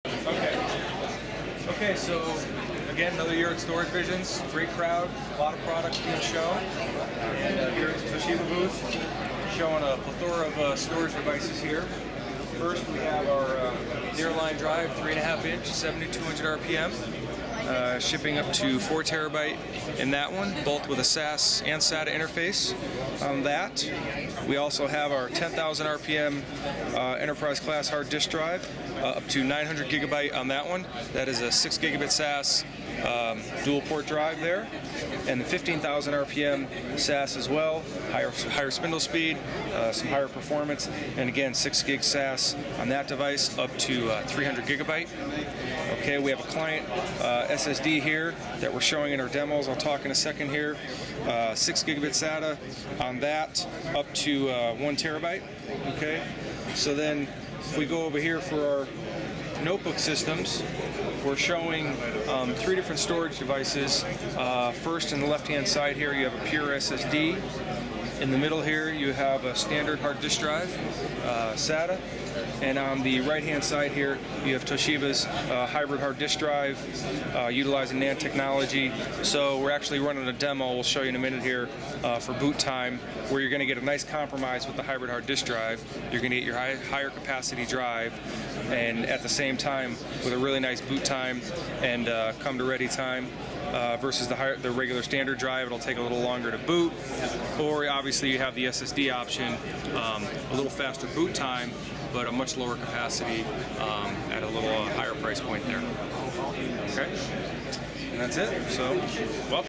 CES 2013 - Toshiba storage video from CES 2013 discuss its 4TB hard drive and 15,000 RPM 2.5" HDD's
Toshiba recorded a video with us this week in Las Vegas highlighting the various products in their line, with a near-line 3.5" 7200 RPM 4TB SAS/SATA interface HDD along with their 10,000 RPM enterprise hard disk drive that has up to 900GB of capacity in a 2.5 in form factor.